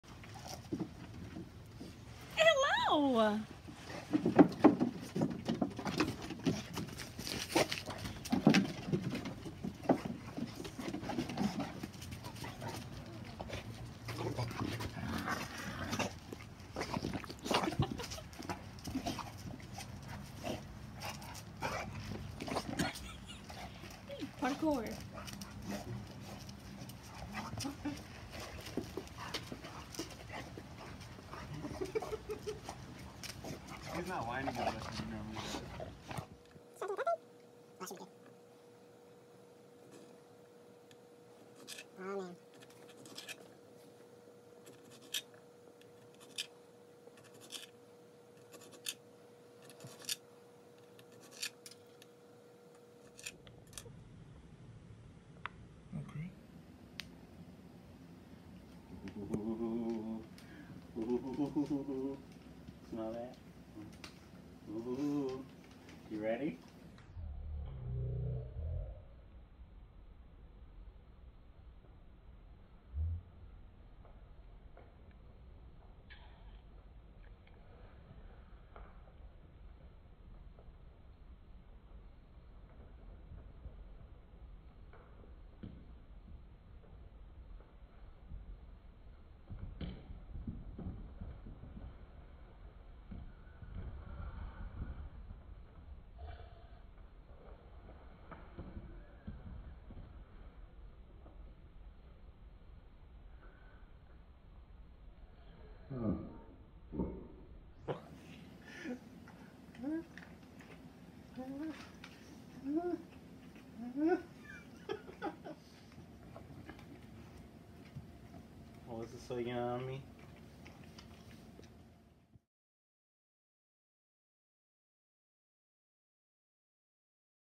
Dumping a bag of treats on my English bulldog
Please enjoy the cute video of my husband and I dumping a bag of treats on my dog. I made a Facebook post saying if I got 100 comments I would do it.